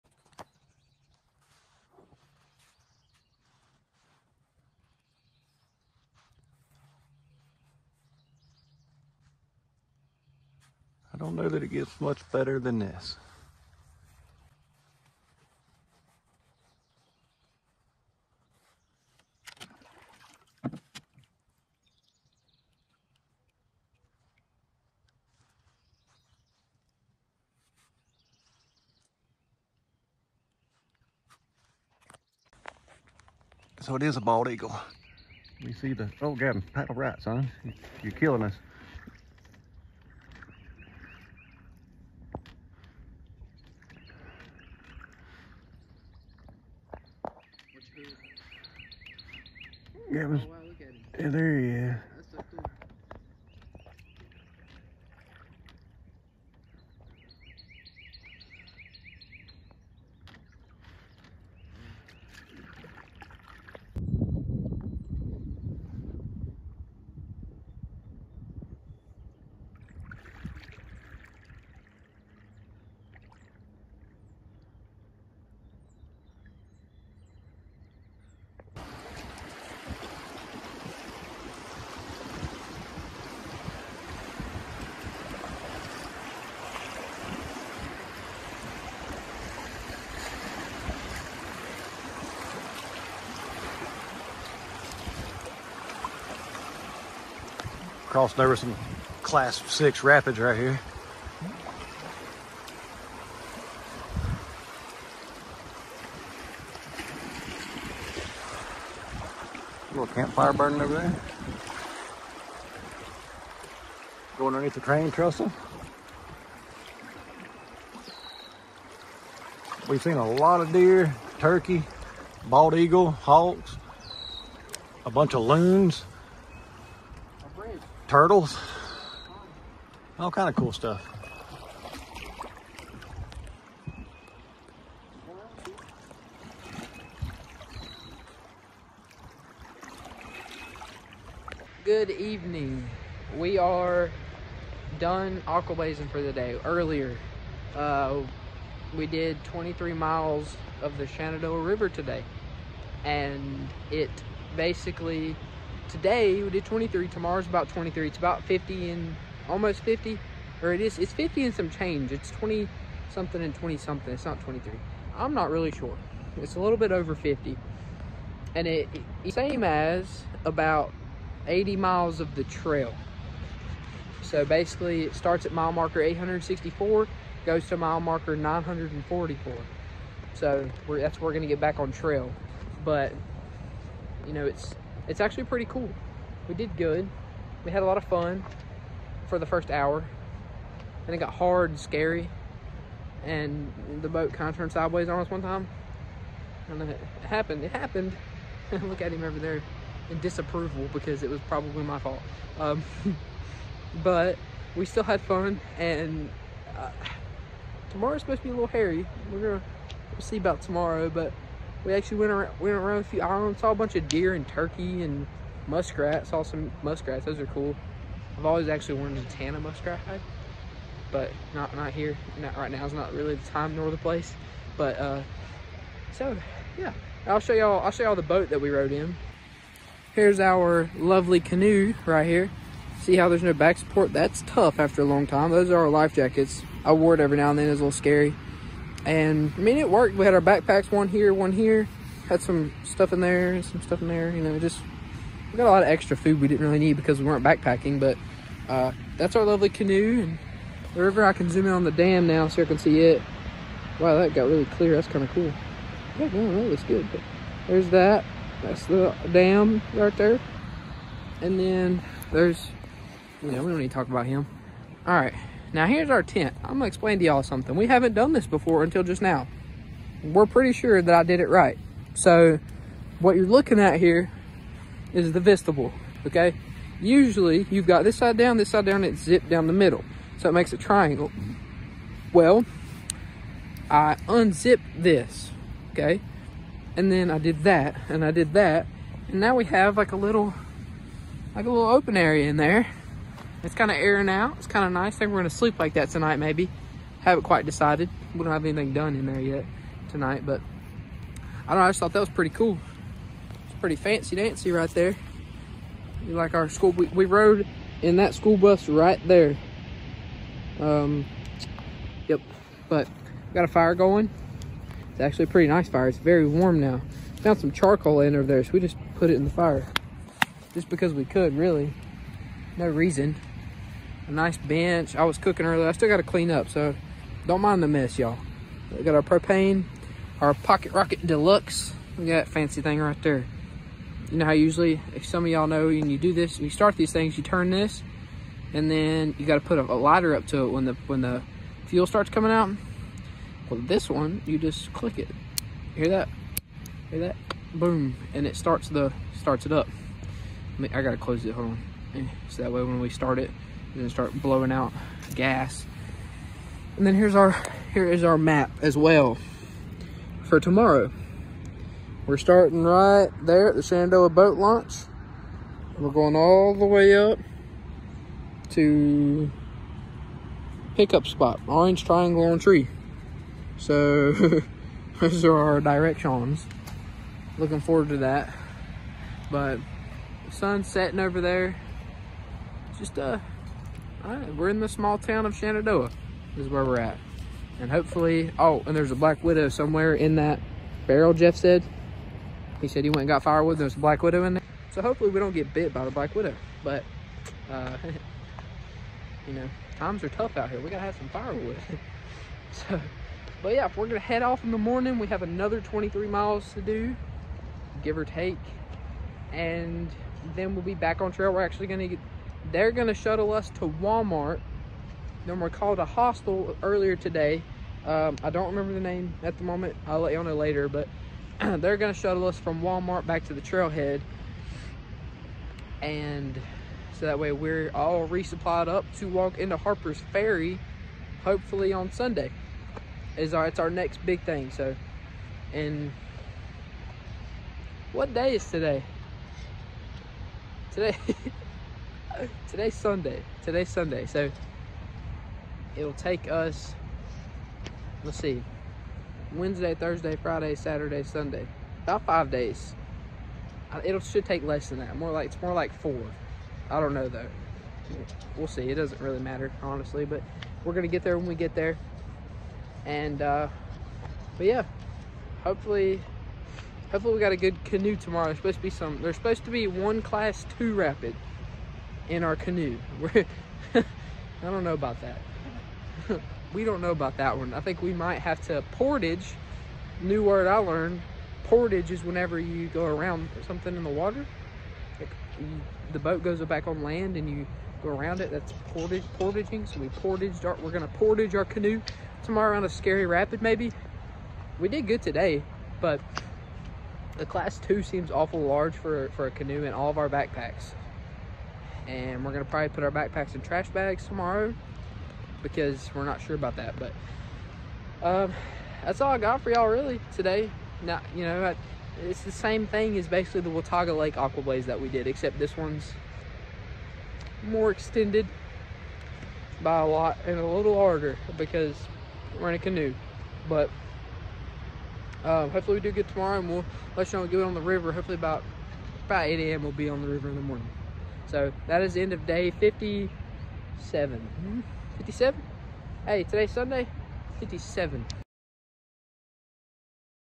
Day 57 Thru Hiking The Appalachian Trail - Aqua Blaze
Today we spent on the Shenandoah River Aqua blazing instead of hiking.

It was a good break from backpacking. We saw so many animals and had a great evening by the campfire.

Excited for tomorrow.